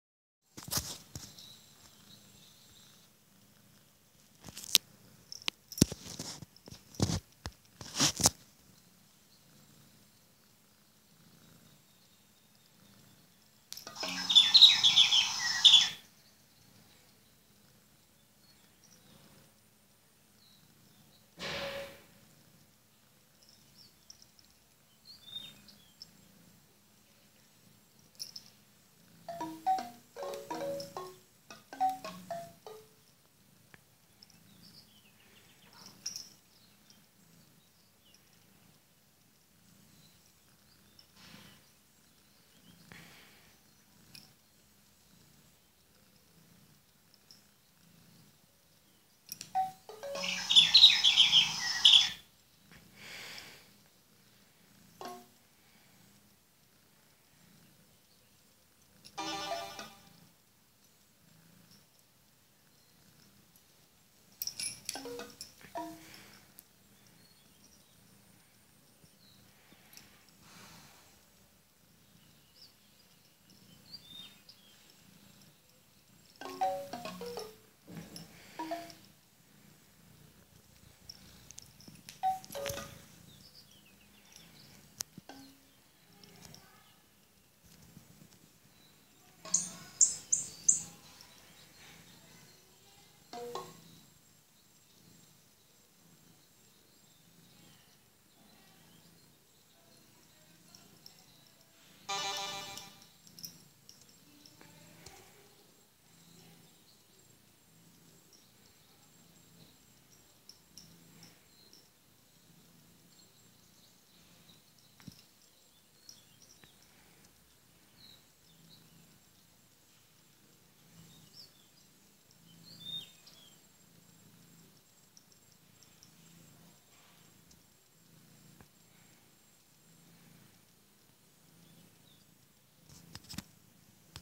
Gatinhas jogando no tablet
Este vídeo não é da Luara, mas de alguns dos meus gatos jogando no tablet... Elas adoraram